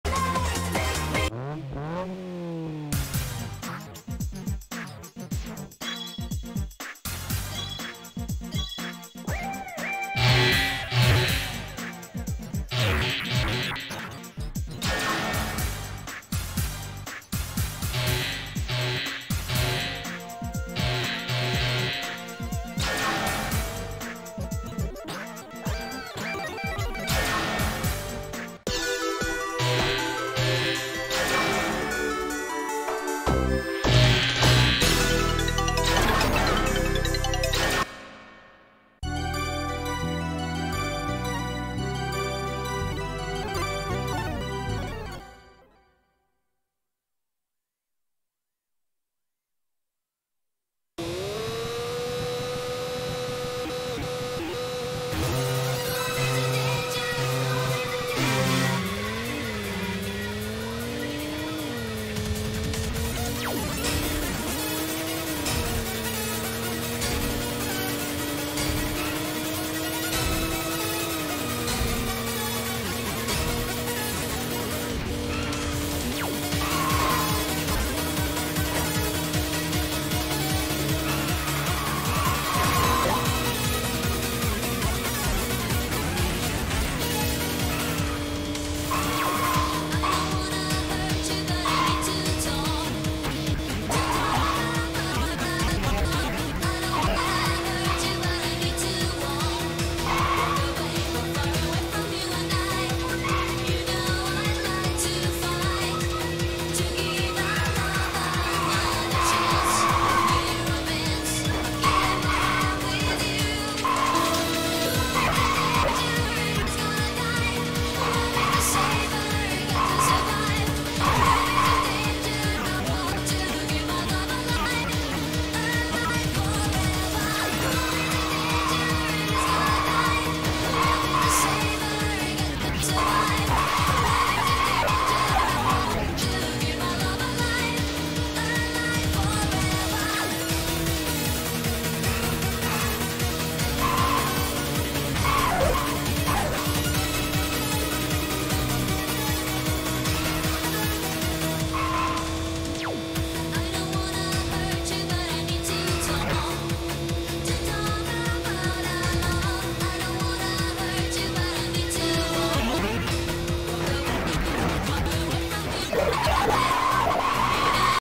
Demul having a stroke
Video recording by derole.
If someone knows how to replicate this please tell me. derole doesn't know how he did it and I want to play the game like this just for the amusement of it.